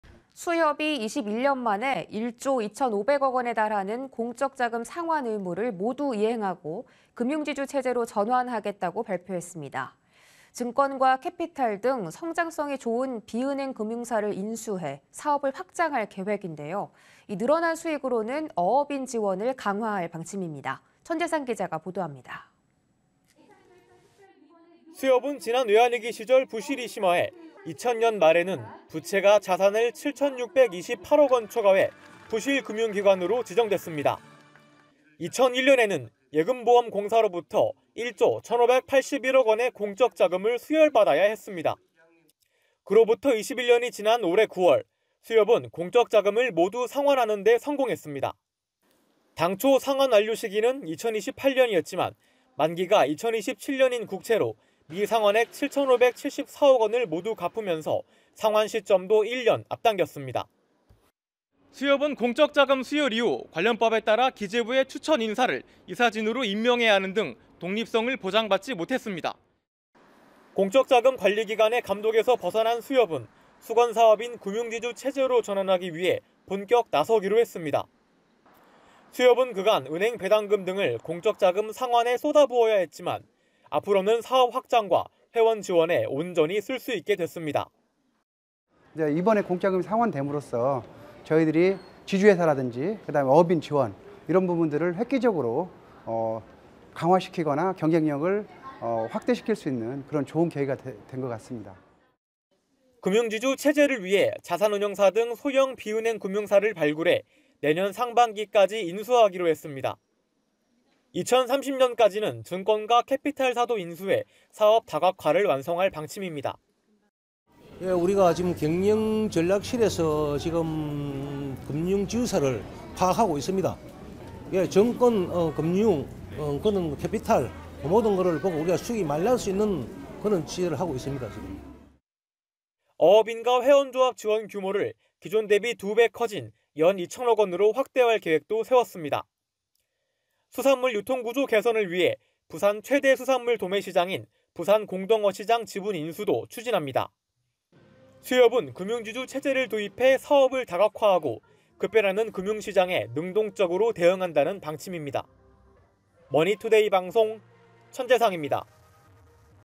수협, 21년만에 '공적자금' 딱지떼고 '금융지주' 도약 / 머니투데이방송 (뉴스)
11/23 MTN 핫라인 5

수협이 21년만에 1조2500억원에 달하는 공적자금 상환 의무를 모두 이행하고, 금융지주 체제로 전환하겠다고 발표했습니다. 증권과 캐피탈 등 성장성이 좋은 비은행 금융사를 인수해 사업을 확장할 계획인데요. 늘어난 수익으로는 어업인 지원을 강화할 방침입니다. 천재상 기자가 보도합니다.